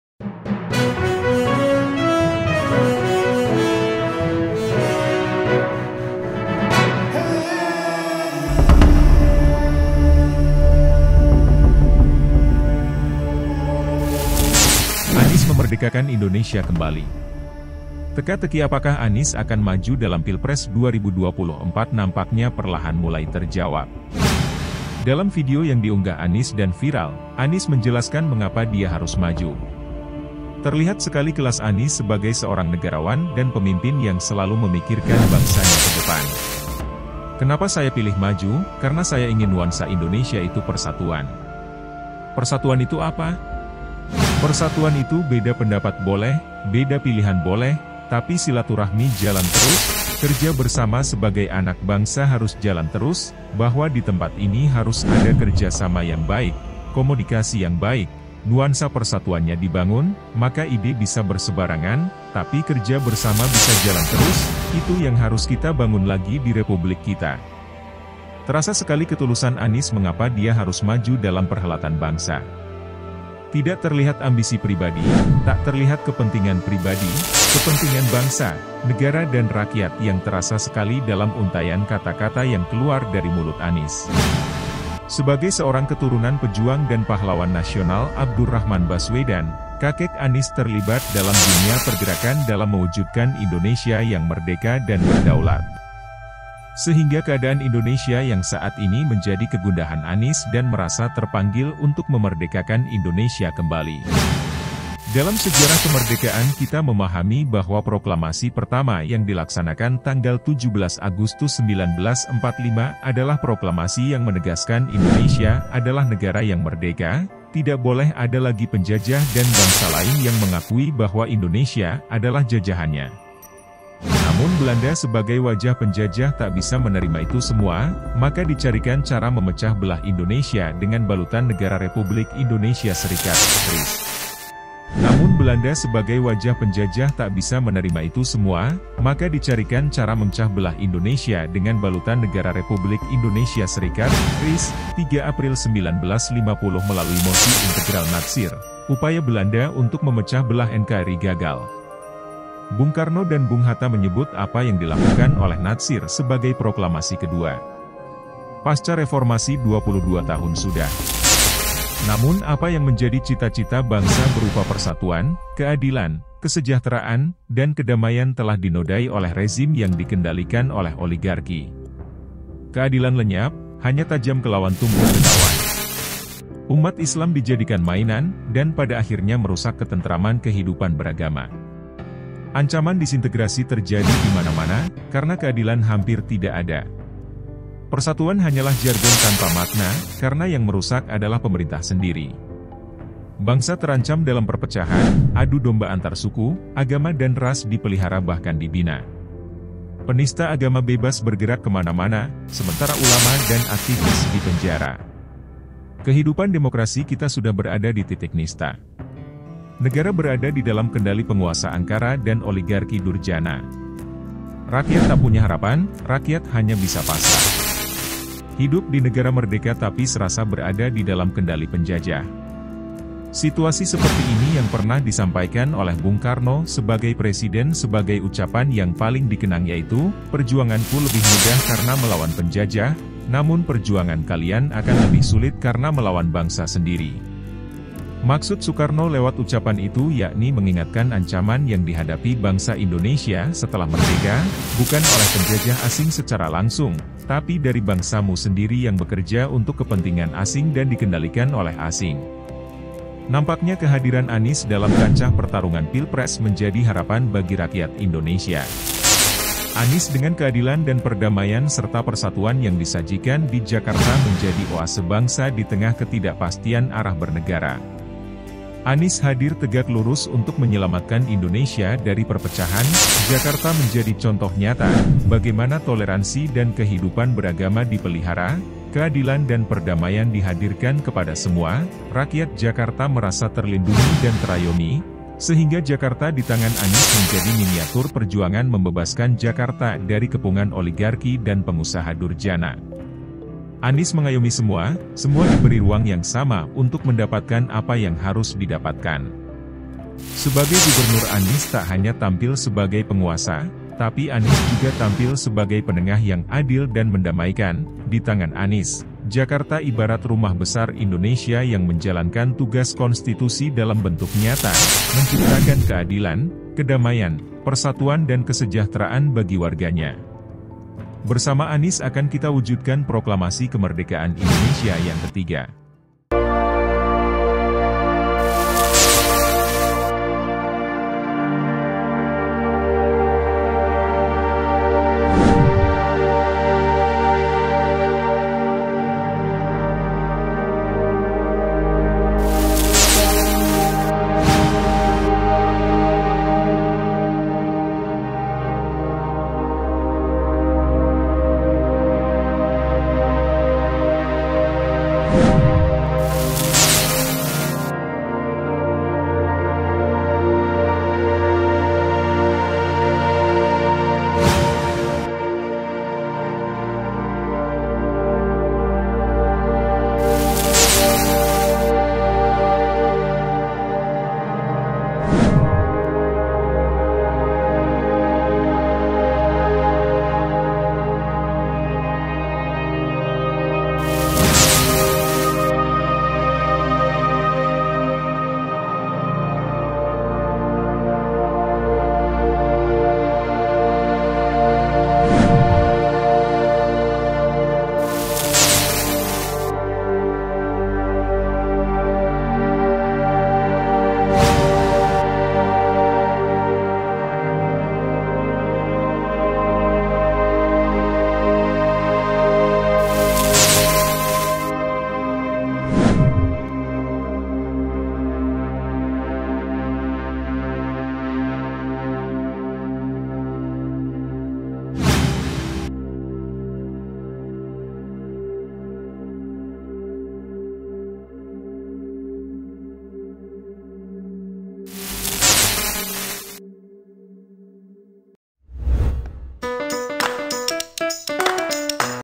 Anies Memerdekakan Indonesia Kembali
Penista agama bebas bergerak kemana-mana, sementara ulama dan aktivis di penjara. Kehidupan demokrasi kita sudah berada di titik nista.